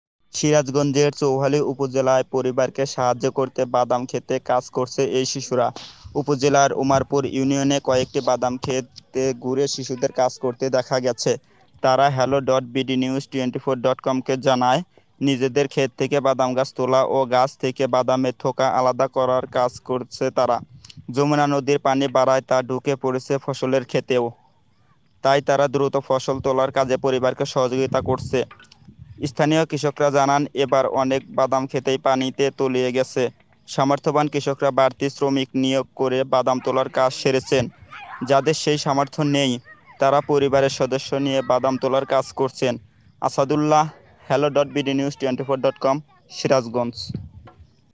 যমুনার চরে বাদাম তোলার কাজে শিশুরা
সিরাজগঞ্জের চৌহালী উপজেলায় বাদাম ক্ষেতে কাজ করে পরিবারকে সাহায্য করছে শিশুরা।